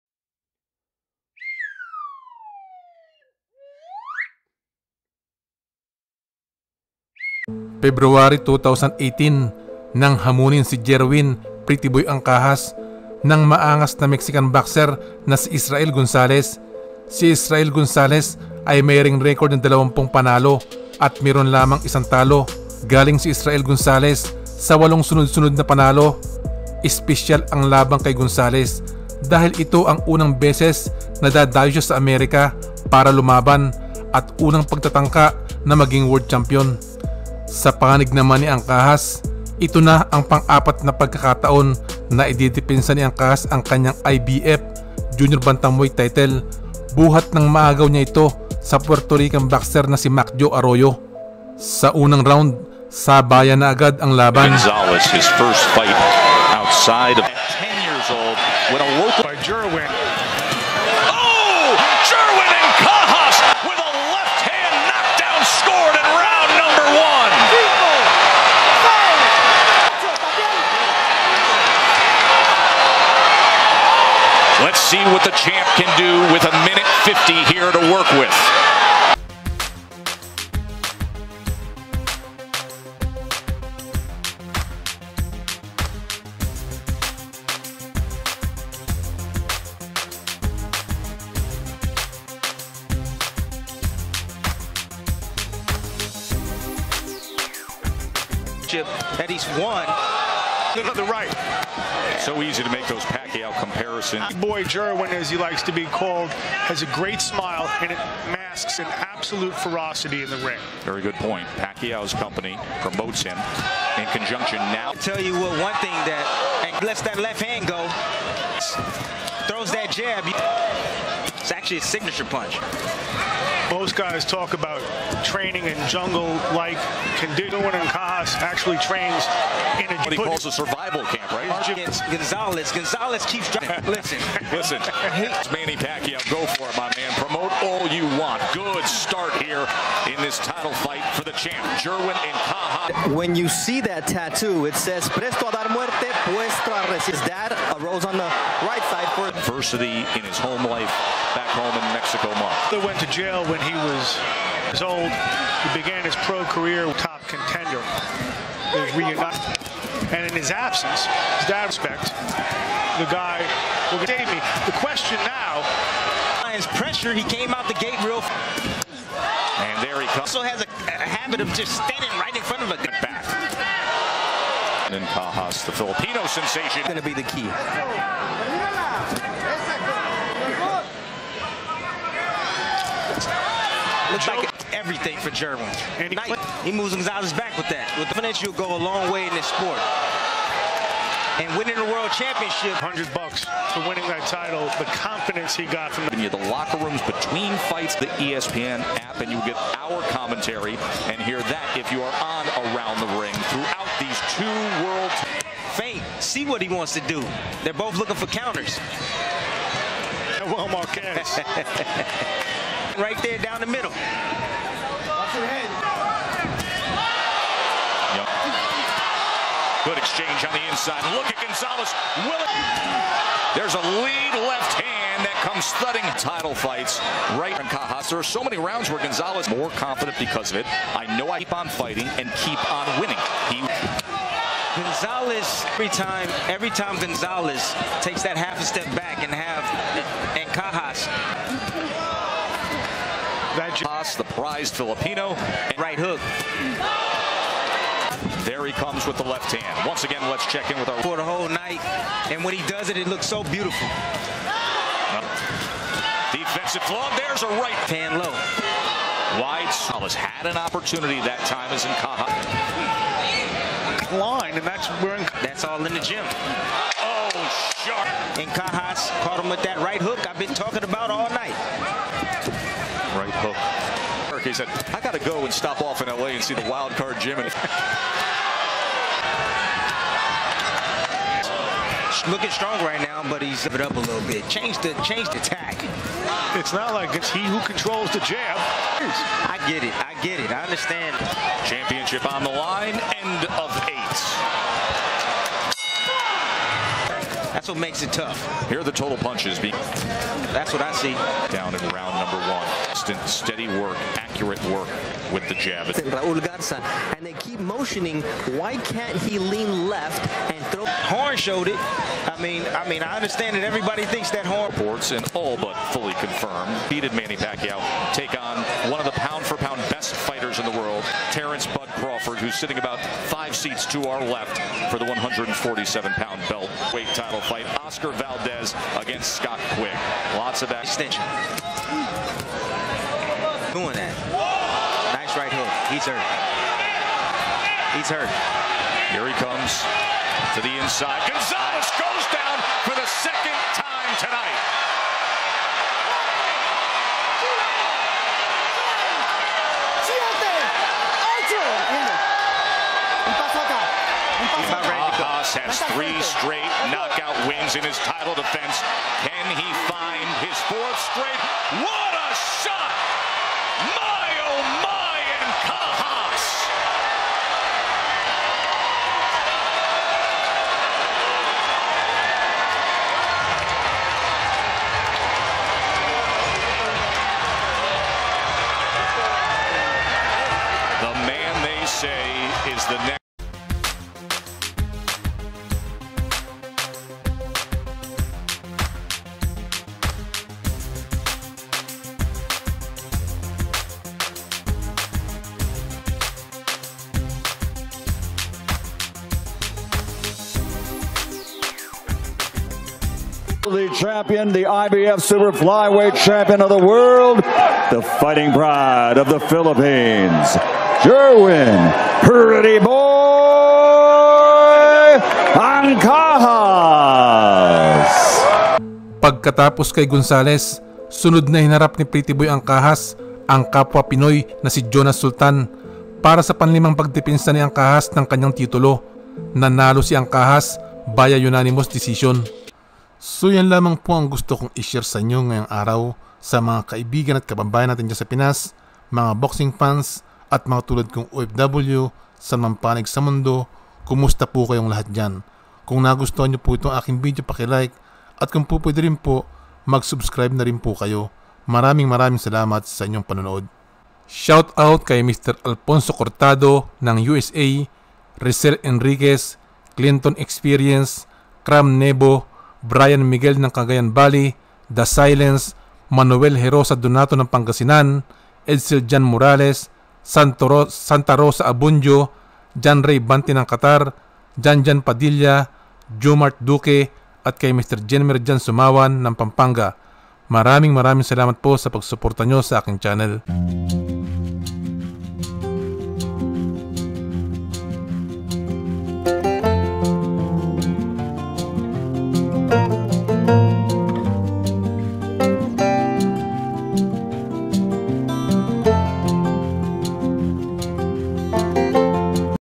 DUROG kay PRETTY BOY ANCAJAS!
Boxing match highlights between Jerwin "Pretty Boy Ancajas" of the Philippines and Israel Gonzalez of Mexico. The match was held in Texas, USA  on February 3, 20218.